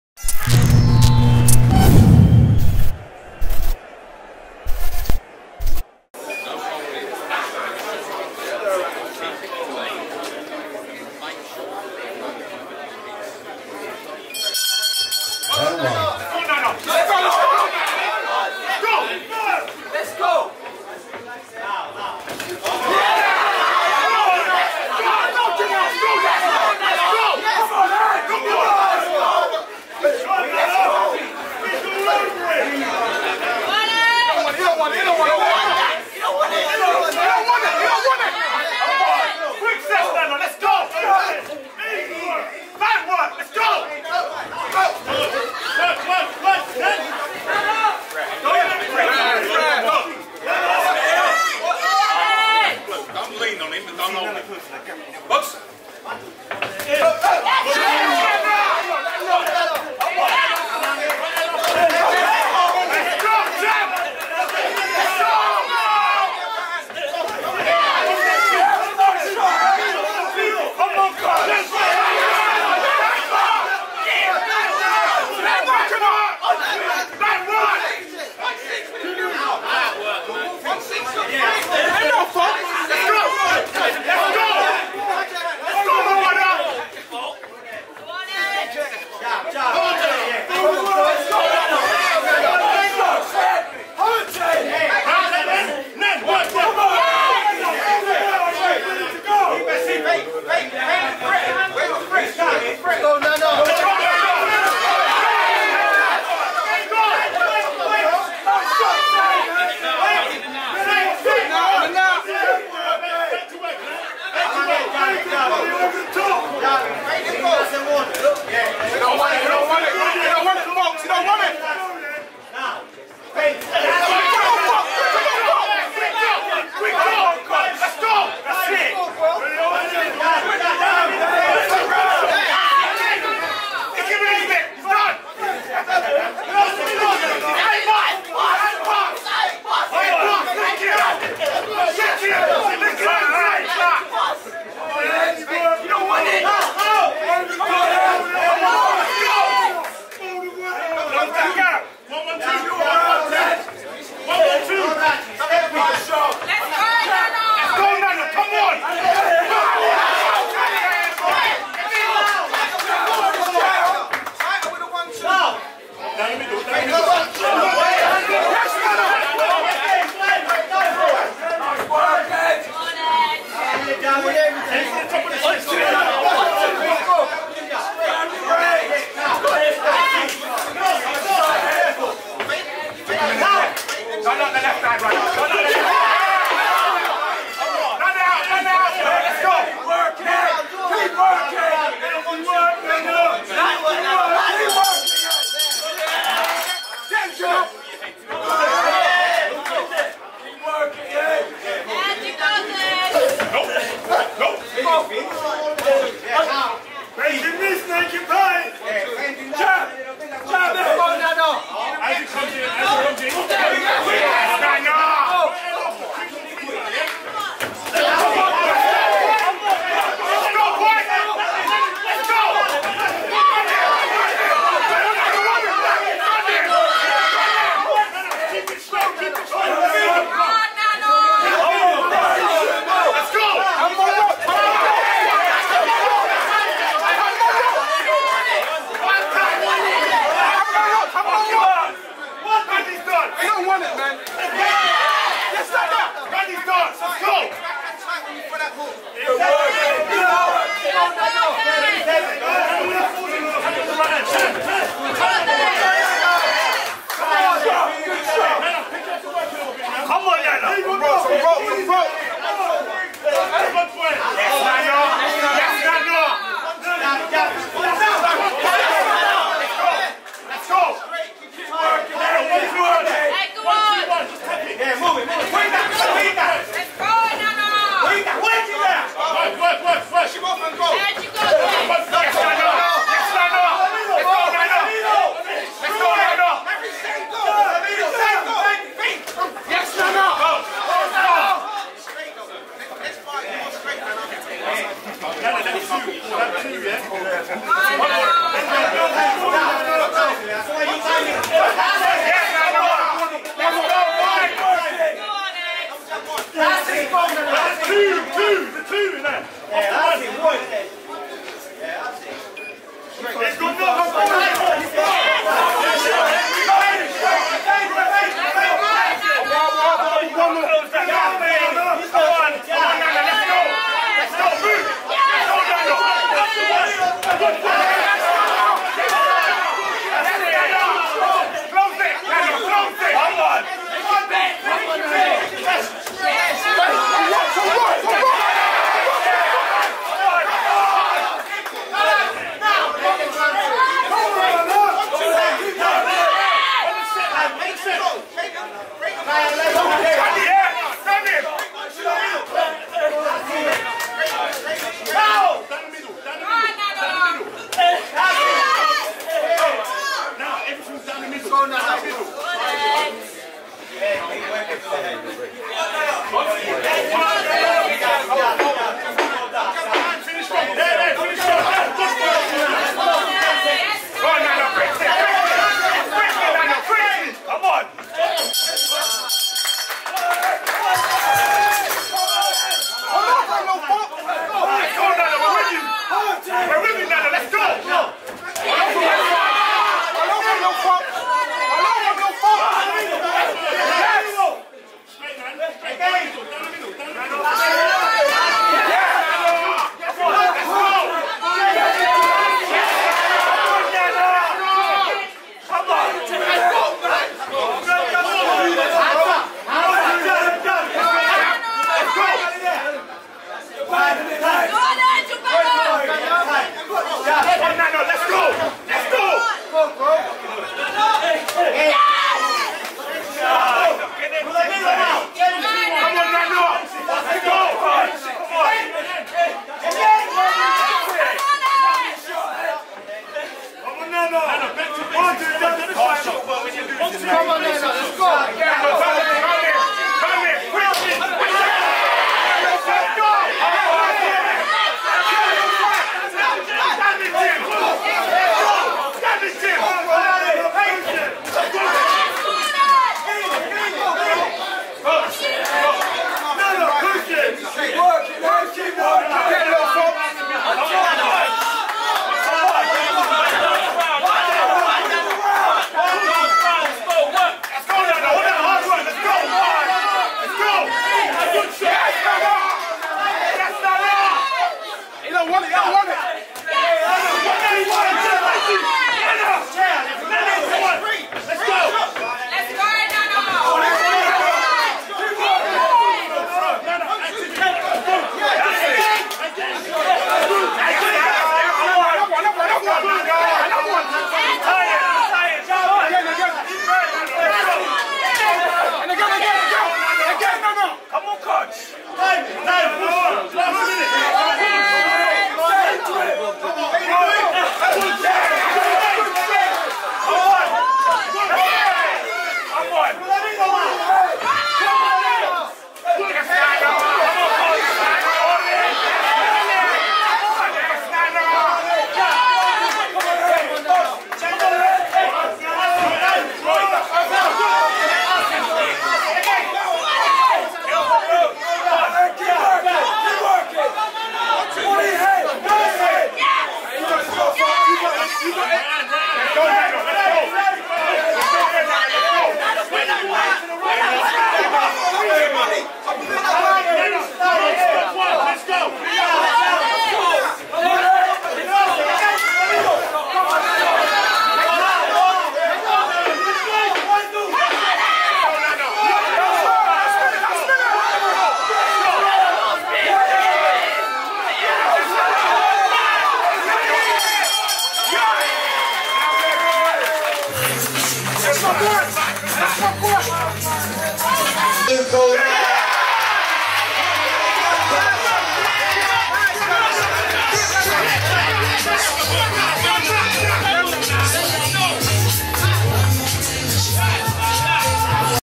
Edmund Chattey vs Nana Donkor Belvedere Boxing Club Event (London 23/10/2021)
Edmund Chattey (Cuban Boxing Academy) vs Nana Donkor (Fight Hub Felbridge BC)
3 x 3 minute rounds.
Official England Boxing Bout.